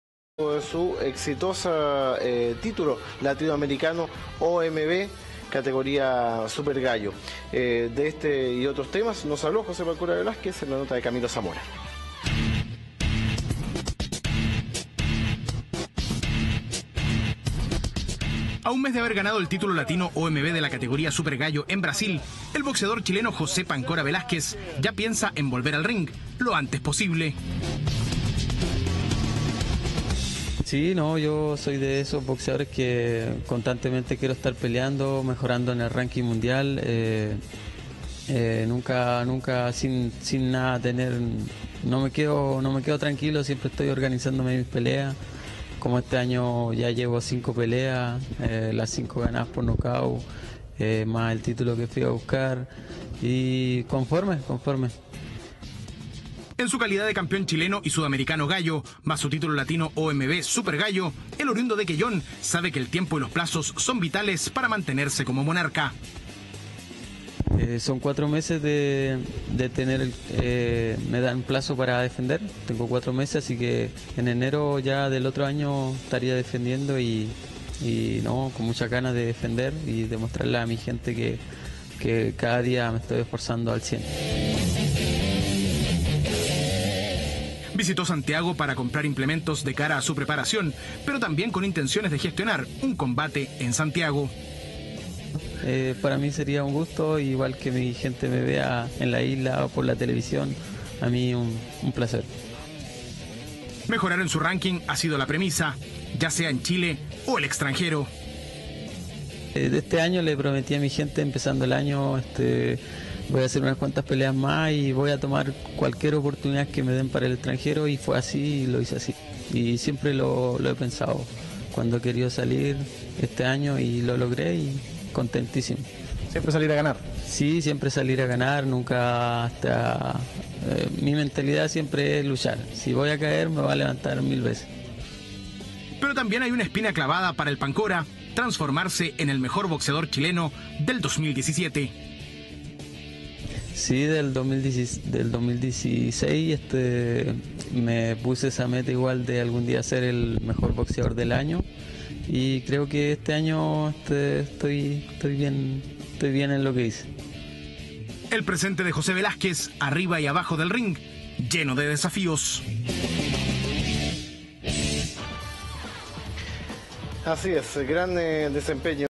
José "Pancora" Velásquez quiere volver al ring tras ganar Título Latino OMB
El oriundo de Quellón busca subir al ring nuevamente para subir en el ránking mundial y competir por un campeonato del mundo.